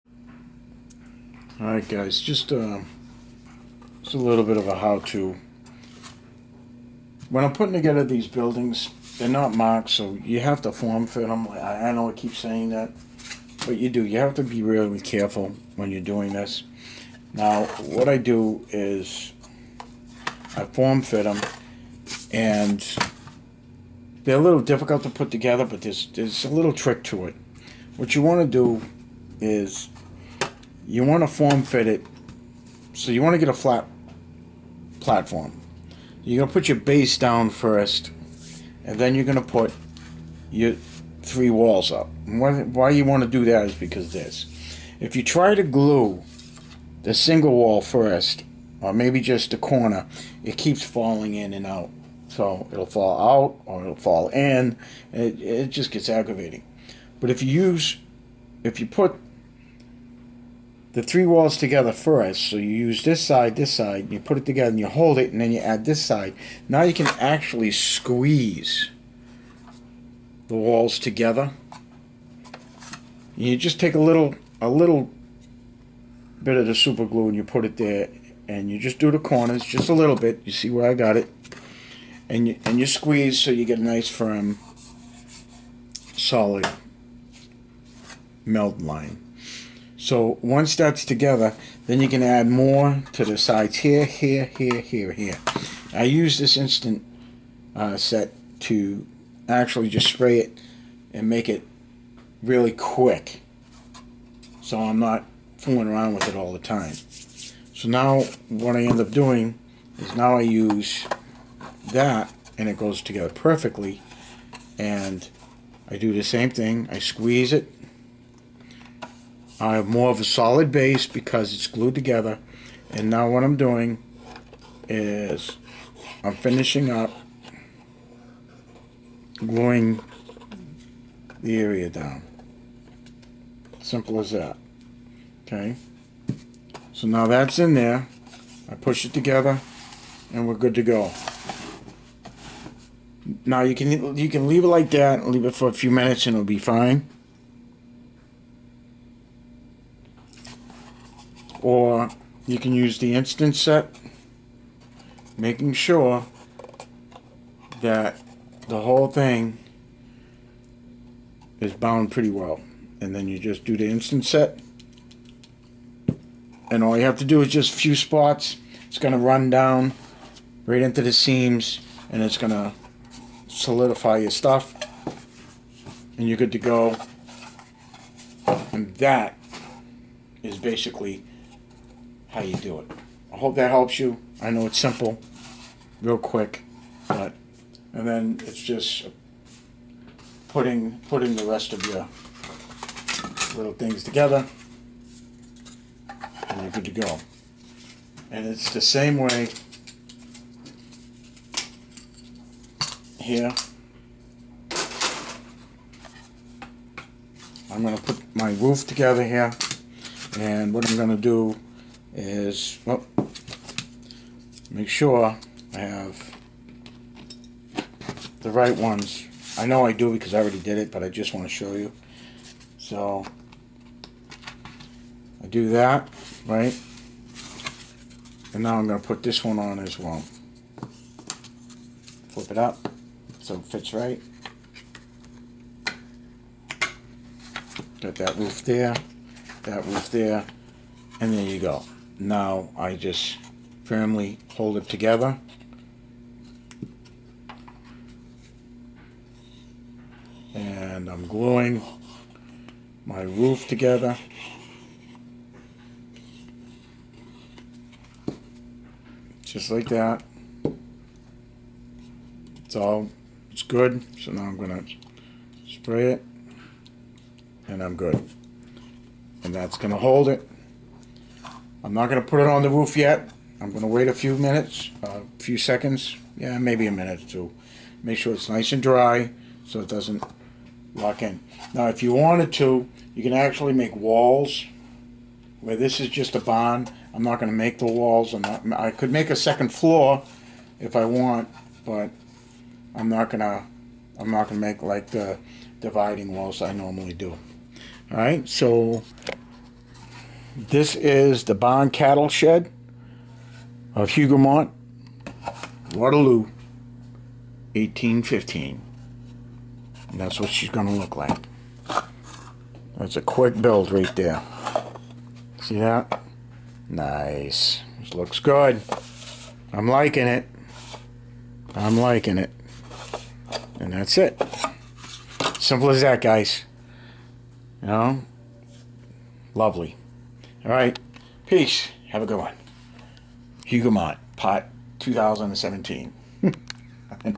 Bob's From the Basement 5/9/18: Hougomont 28mm How to build the Barn Cattle Shed
A quick how I put together the MDF wooden Barn Cattle Shed from the 28MM Sarrissa Hougoumont laser cut wooden farm house for Waterloo.